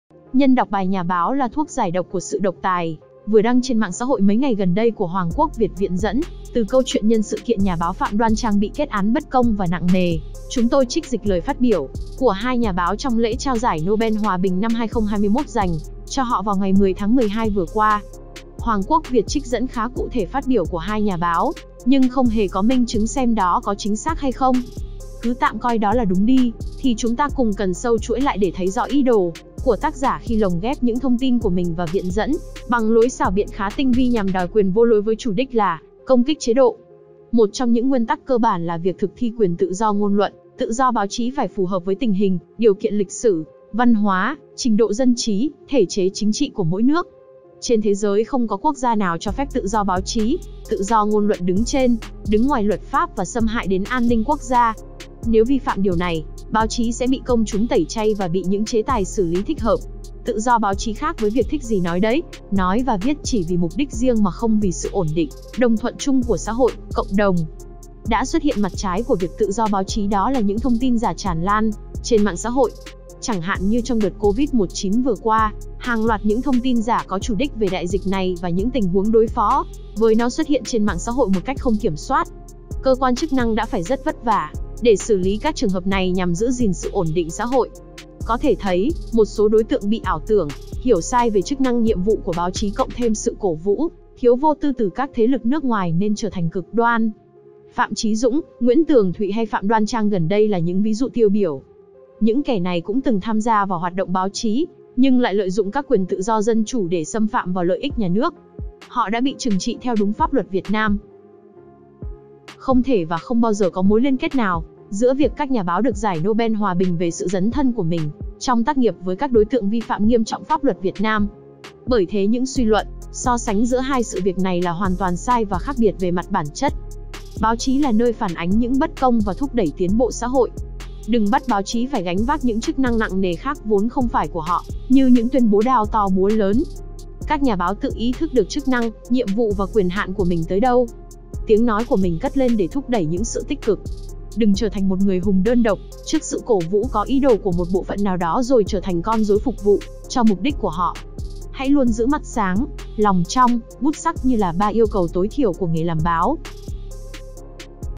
Đừng hiểu lầm về chức năng của nhà báo
Nhân đọc bài “Nhà báo là thuốc giải độc của sự độc tài” vừa đăng trên mạng xã hội mấy ngày gần đây của Hoàng Quốc Việt viện dẫn từ câu chuyện “Nhân sự kiện nhà báo Phạm Đoan Trang bị kết án bất công và nặng nề, chúng tôi trích dịch lời phát biểu của hai nhà báo trong lễ trao giải Nobel Hòa bình năm 2021 dành cho họ vào ngày 10 tháng 12 vừa qua”. Hoàng Quốc Việt trích dẫn khá cụ thể phát biểu của 2 nhà báo, nhưng không hề có minh chứng xem đó có chính xác hay không. Cứ tạm coi đó là đúng đi, thì chúng ta cùng cần xâu chuỗi lại để thấy rõ ý đồ của tác giả khi lồng ghép những thông tin của mình và viện dẫn bằng lối xảo biện khá tinh vi nhằm đòi quyền vô lối với chủ đích là công kích chế độ.

Nhân đây tôi có vài chia sẻ, trao đổi để mọi người hiểu rõ vấn đề và tránh bị dụ dẫn bởi chiêu trò khá hiểm của Hoàng Quốc Việt.

Một trong những nguyên tắc cơ bản là việc thực thi quyền tự do ngôn luận, tự do báo chí phải phù hợp với tình hình, điều kiện lịch sử, văn hóa, trình độ dân trí, thể chế chính trị của mỗi nước.

 Điều 11, Tuyên ngôn Nhân quyền và Dân quyền năm 1789 của nước Pháp đã quy định: “Tự do trao đổi suy nghĩ và ý kiến là một trong những quyền quý giá nhất của con người. Vì thế, bất kỳ công dân nào cũng có thể nói, viết và công bố tự do; tuy nhiên, họ sẽ chịu trách nhiệm nếu lạm dụng quyền tự do này theo quy định của pháp luật”. Nước Mỹ tuy không ban hành luật riêng về báo chí, nhưng có nhiều điều luật khác của quốc hội, quy định có tính pháp lý của tòa án cũng đưa ra những giới hạn nhất định đối với báo chí, đối với quyền và trách nhiệm của công dân liên quan đến báo chí nhằm tránh xâm hại đến an ninh quốc gia. Trên thế giới không có quốc gia nào cho phép tự do báo chí, tự do ngôn luận “đứng trên, đứng ngoài” luật pháp và xâm hại đến an ninh quốc gia. Nếu vi phạm điều này, báo chí sẽ bị công chúng tẩy chay và bị những chế tài xử lý thích hợp.

Tự do báo chí khác với việc thích gì nói đấy, nói và viết chỉ vì mục đích riêng mà không vì sự ổn định, đồng thuận chung của xã hội, cộng đồng. Đã xuất hiện mặt trái của việc tự do báo chí đó là những thông tin giả tràn lan trên mạng xã hội. Chẳng hạn như trong đợt Covid-19 vừa qua, hàng loạt những thông tin giả có chủ đích về đại dịch này và những tình huống đối phó với nó xuất hiện trên mạng xã hội một cách không kiểm soát. Cơ quan chức năng đã phải rất vất vả để xử lý các trường hợp này nhằm giữ gìn sự ổn định xã hội.

Như đã nói ở trên, việc thực thi quyền tự do báo chí, tự do ngôn luận phải phù hợp với đặc điểm tình hình mỗi nước. Một số đối tượng bị ảo tưởng, hiểu sai về chức năng nhiệm vụ của báo chí cộng thêm sự “cổ vũ” thiếu vô tư từ các thế lực nước ngoài nên trở thành cực đoan. Phạm Chí Dũng, Nguyễn Tường Thụy hay Phạm Đoan Trang gần đây là những ví dụ tiêu biểu. Những kẻ này cũng từng tham gia vào hoạt động báo chí nhưng lại lợi dụng các quyền tự do dân chủ để xâm phạm vào lợi ích nhà nước. Họ đã bị trừng trị theo đúng pháp luật Việt Nam.

Không thể và không bao giờ có mối liên kết nào giữa việc các nhà báo được giải Nobel hòa bình về sự dấn thân của mình trong tác nghiệp với các đối tượng vi phạm nghiêm trọng pháp luật Việt Nam. Bởi thế những suy luận, so sánh giữa hai sự việc này là hoàn toàn sai và khác biệt về mặt bản chất.

Bình luận trước thông tin hai nhà báo trong đó có ông Muratov, một Tổng biên tập báo ở Nga, được giải Nobel Hòa bình, điện Kremly từ nước Nga bình luận: “Chúng tôi chúc mừng ông Dmitry Muratov. Ông đã kiên trì làm việc theo lý tưởng của riêng mình, đã cống hiến cho những lý tưởng đó. Ông là một người tài năng và dũng cảm”.

Tuy nhiên, Tổng thống Nga Putin cảnh báo nhà báo Dmitry Muratov không nên cố gắng núp sau giải Nobel Hòa bình và dùng nó làm lá chắn để vi phạm pháp luật Nga, cũng như thu hút sự chú ý vào bản thân.

Cũng nên rạch ròi như thế, đừng để các nhà báo quá ảo tưởng vào bản thân và ảo tưởng vào cái gọi là “quyền lực thứ 4”. Báo chí là nơi phản ánh những bất công và thúc đẩy tiến bộ xã hội. Đừng bắt báo chí phải gánh vác những chức năng nặng nề khác vốn không phải của họ như những tuyên bố “đao to búa lớn”.

Các nhà báo tự ý thức được chức năng, nhiệm vụ và quyền hạn của mình tới đâu. Tiếng nói của mình cất lên để thúc đẩy những sự tích cực. Đừng trở thành một người hùng đơn độc trước sự cổ vũ có ý đồ của một bộ phận nào đó rồi trở thành con rối phục vụ cho mục đích của họ.

© Bản quyền thuộc về Online Thanh Tâm, Việt Nam